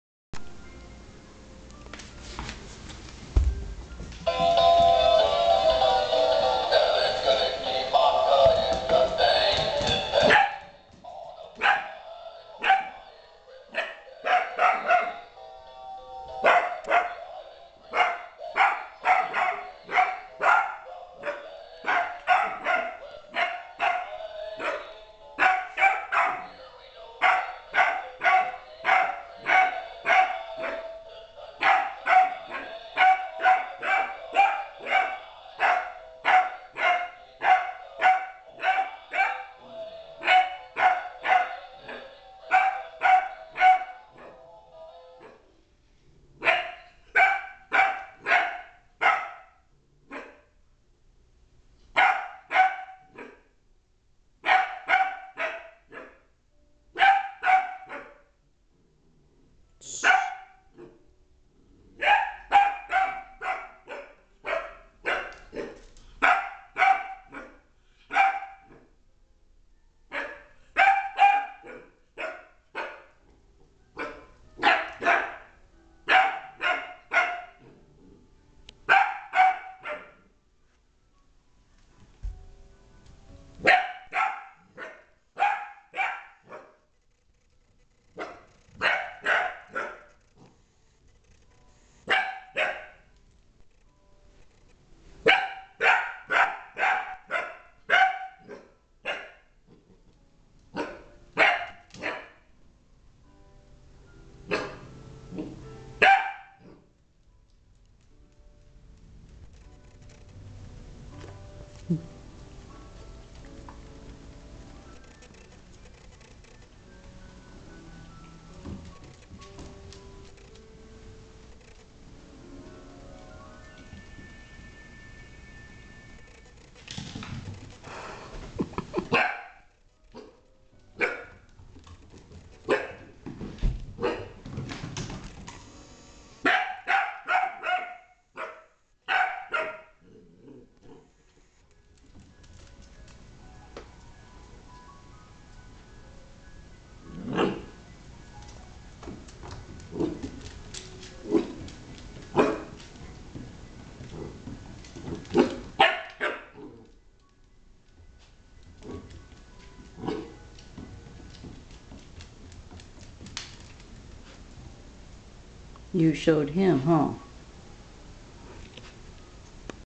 Apollo's first
Apollo, the 13 week old, snoopy-look-a-like dog, in his first home movie and his first meeting with one of his kind... soft of. It might get a little annoying all the barking after a while, but that's what I hear all day long.